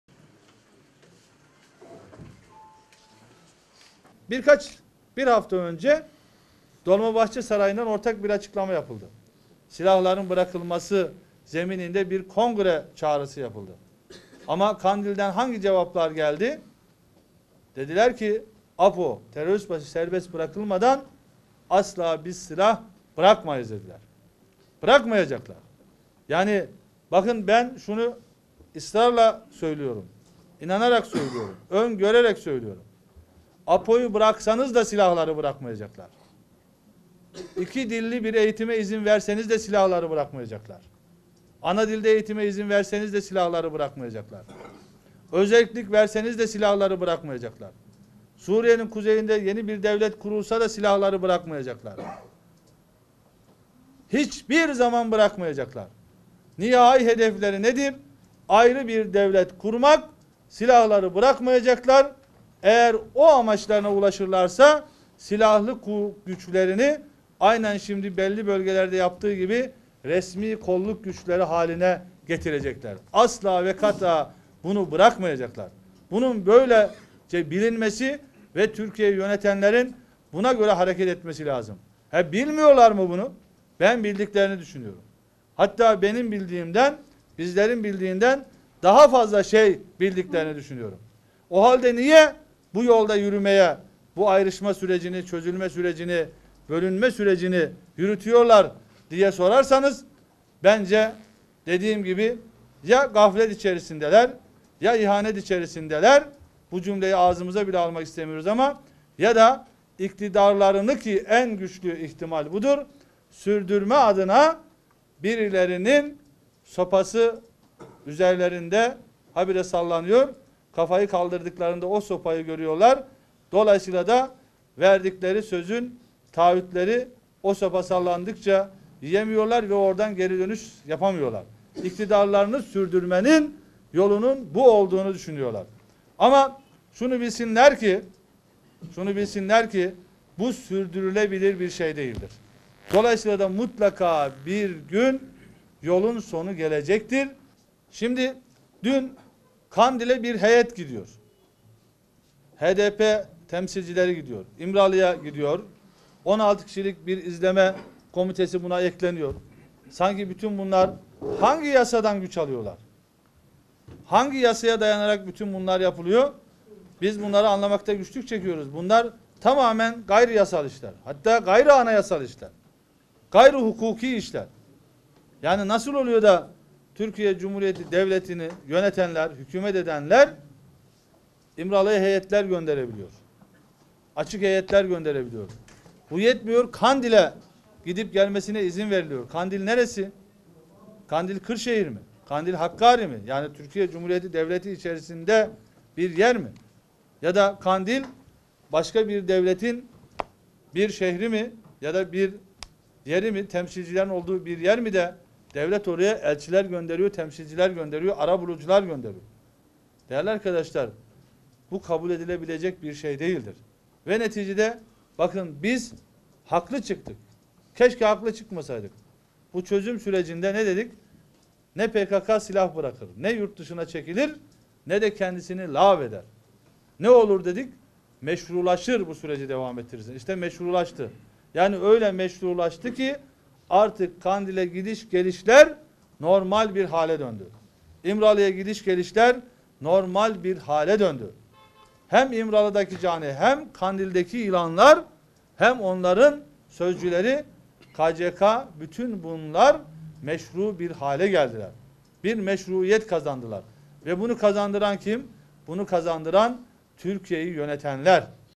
Destici: Apo’yu bıraksanız da silah bırakmayacaklar, hiçbir zaman bırakmayacaklar
Büyük Birlik Partisi (BBP) Genel Başkanı Mustafa Destici, Çözüm süreciyle ilgili değerlendirmelerde bulundu. Kandil ve İmralı’ya heyet gitmesi, HDP temsilcilerinin gitmesinin ve ayrıca 16 kişilik bir izleme komitesinin eklenmesinin gayri anayasal, gayri hukuki işler olduğunu açıkladı.